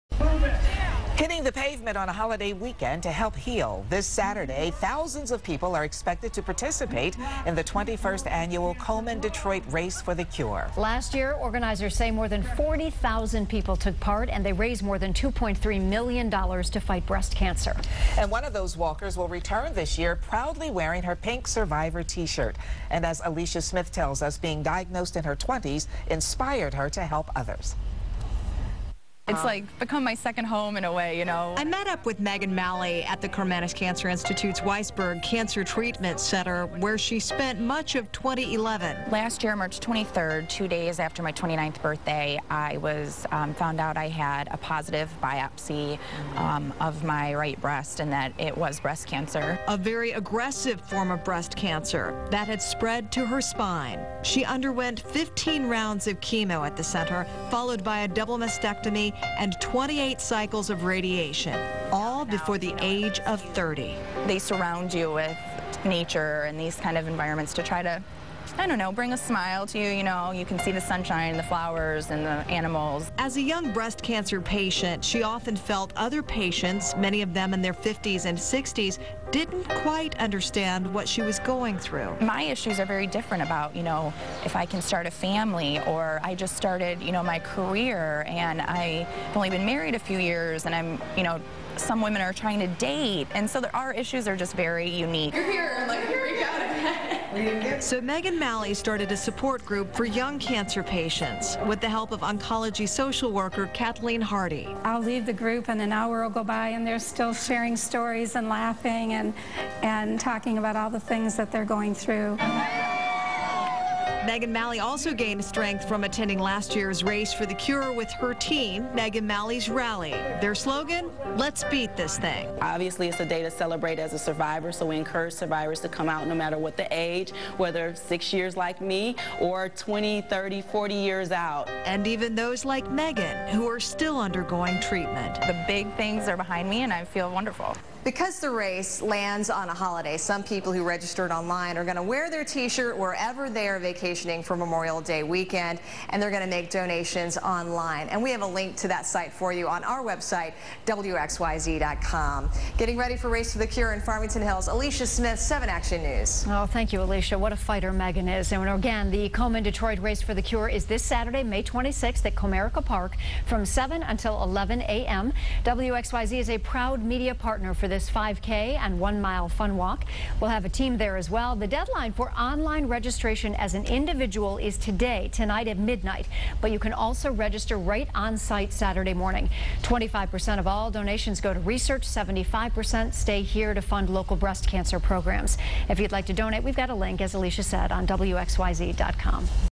Race for the cure this weekend
The annual Race for the Cure is this weekend.